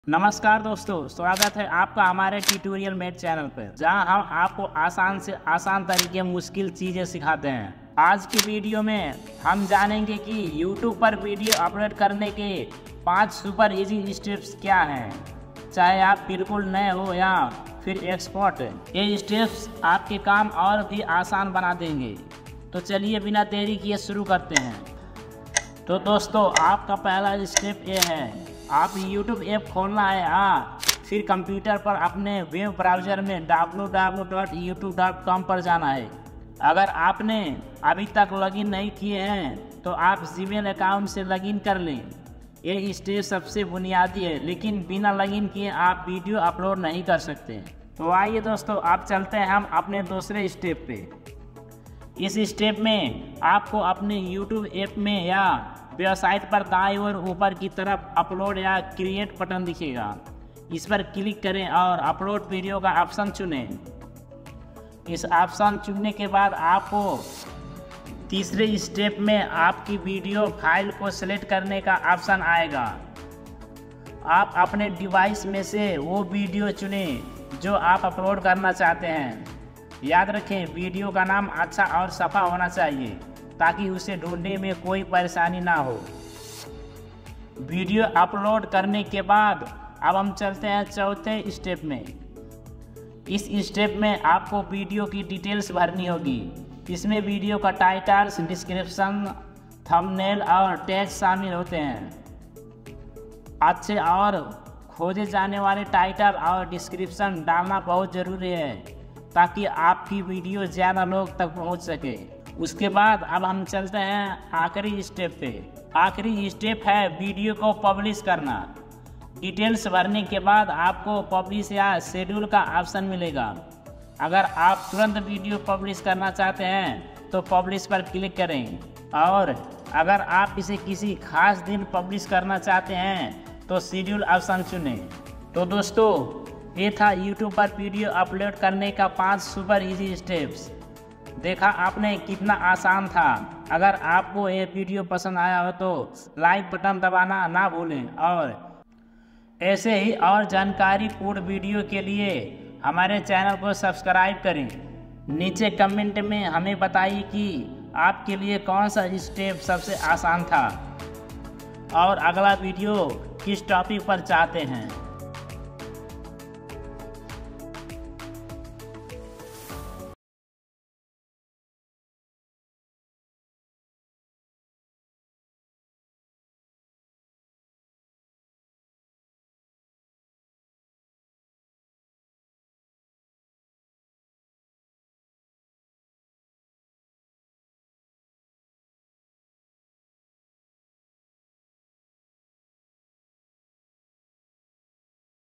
"YouTube पर वीडियो Upload करने के 5 Super easy Step 2024
Want to upload videos on YouTube quickly and easily? In this video, I’ll show you how to upload your YouTube videos in just 5 simple steps! Whether you’re new to YouTube or looking for a faster way to get your content live, this guide is perfect for you. Follow these steps to upload your video and reach your audience effortlessly.